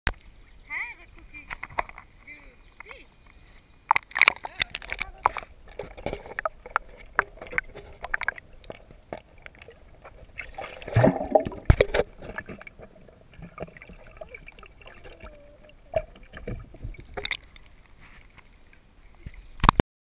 Cookie Cam 002 - Cookie reaching the ramp.
Hehehe in this one is even funnier, he kicks the cam to get out of the water!!